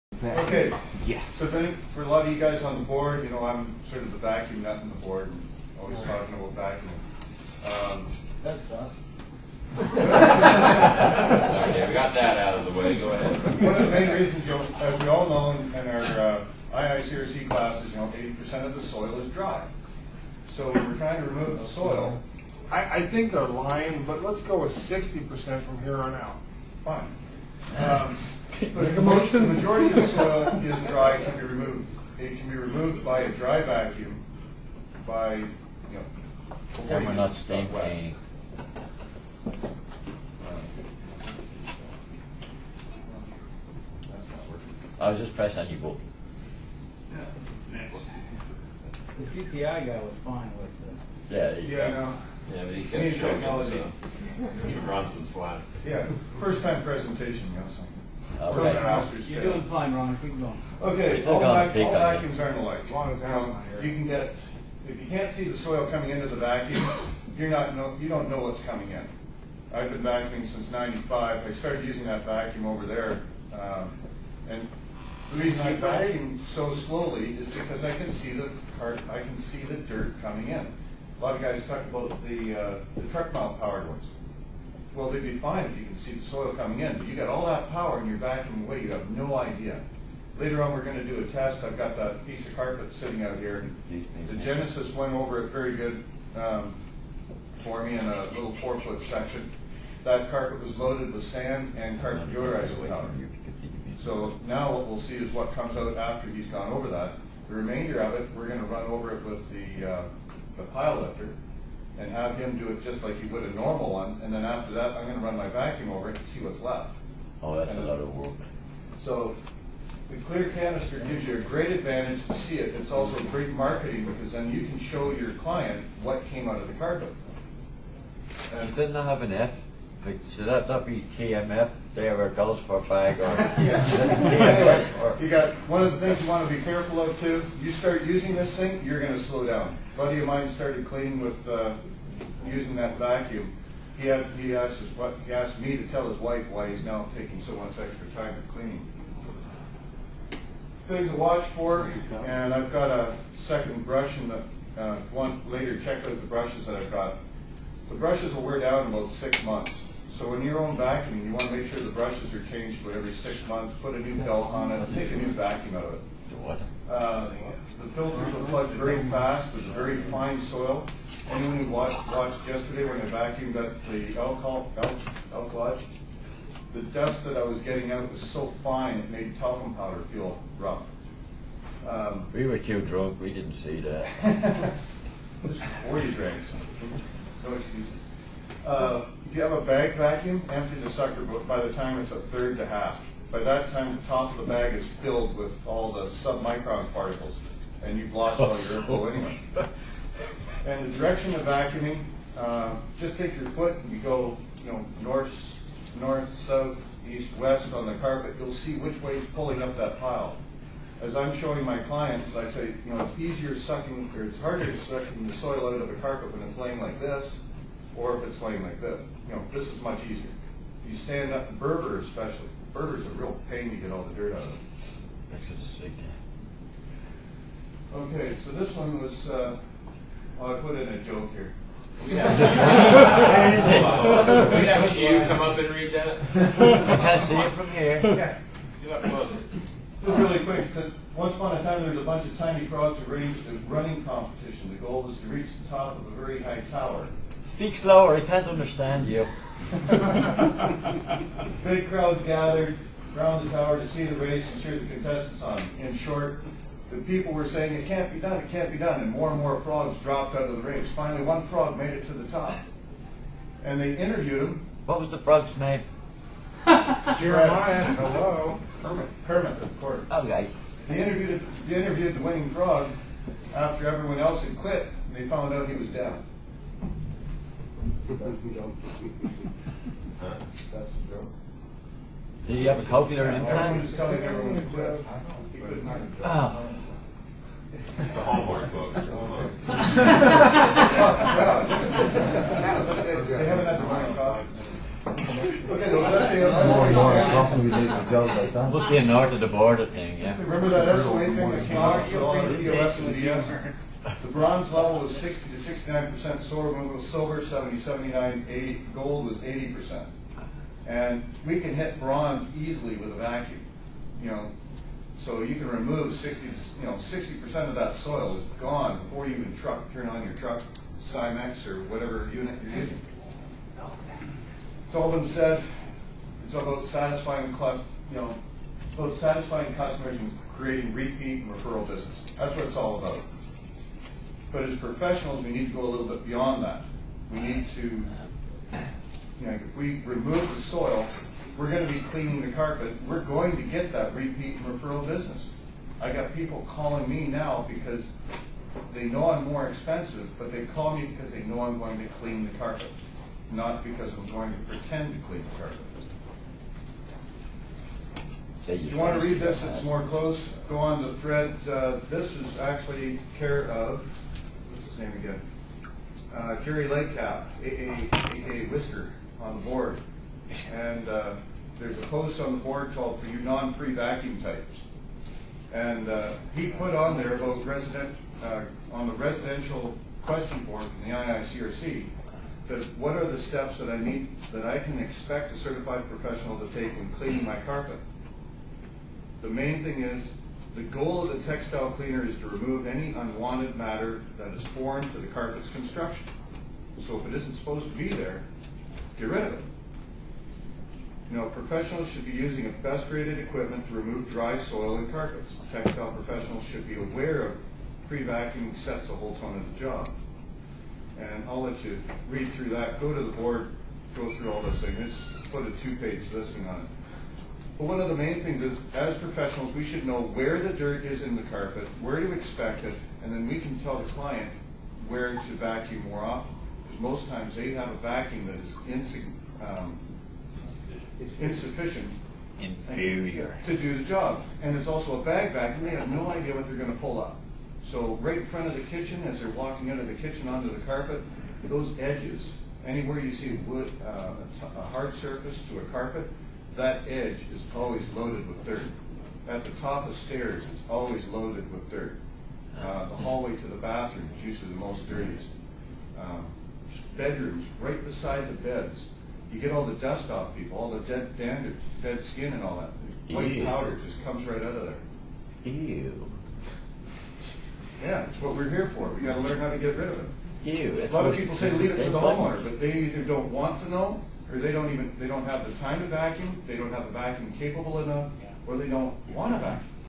Mikefest 09- Vacuuming 101 Pt1
I gave a presentation at Mikefest 5 in San Diego, March 2009, a gathering of top carpet cleaners in Canada and the USA.
The topic was vacuuming.